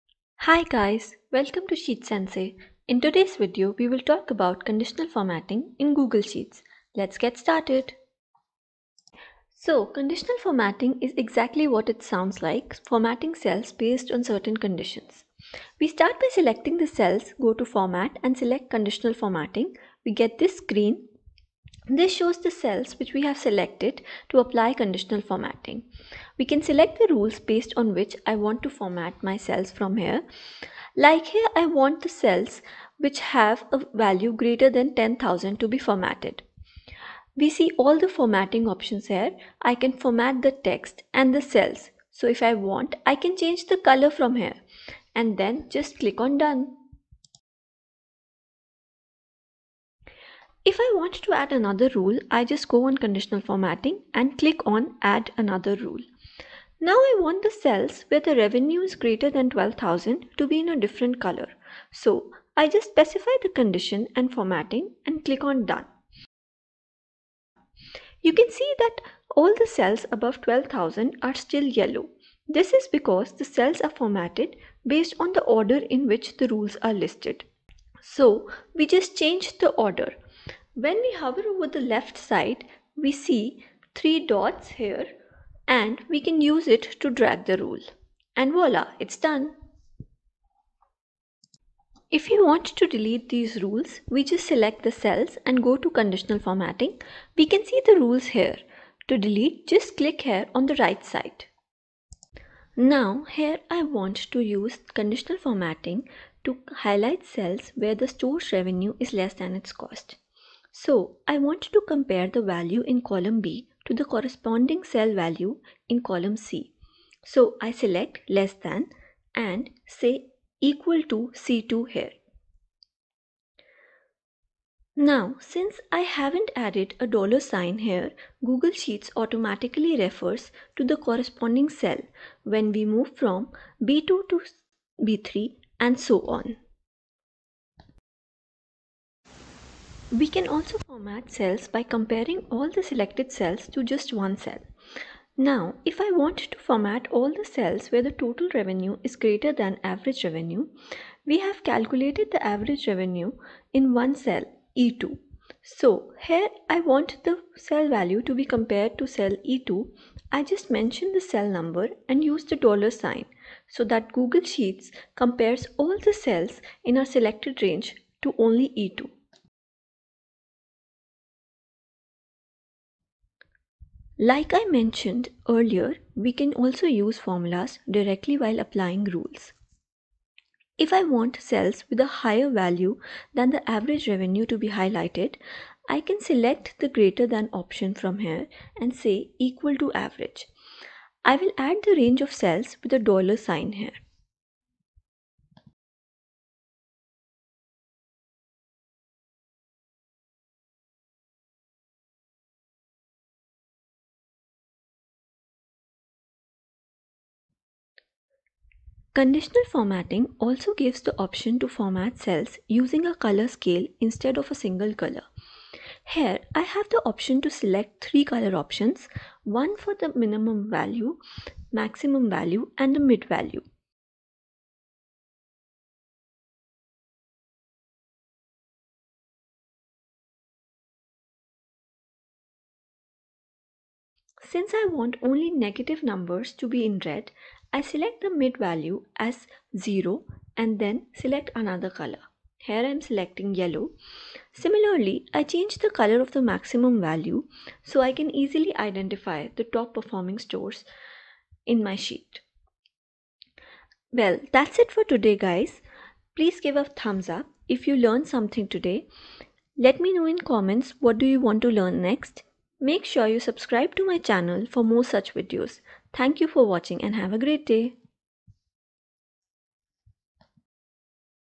Conditional Formatting in Google Sheets: Tutorial 2021| Conditional Formatting: All You Need to Know
Conditional Formatting in Google Sheets: Tutorial 2021| Conditional Formatting: All You Need to Know: Watch this video to learn the fundamentals of conditional formatting in Google Sheets. We start with the basics of conditional formatting and build on to learn more.

00:00 Intro
00:10: Basics - How to format cells using conditional formatting
00:50: Add Multiple Conditions/Rules
01:31: Remove Conditional Formatting
01:43: Conditional Formatting Based on Other Cell Values
02:19: Conditional Formatting Based on a Single Cell's Value
02:58: Using Formulas in Conditional Formatting
03:29: Color Scales in Conditional Formatting

Become a Google Sheet pro and excel at Excel.

Title: Conditional Formatting in Google Sheets: Tutorial 2021| Conditional Formatting: All You Need to Know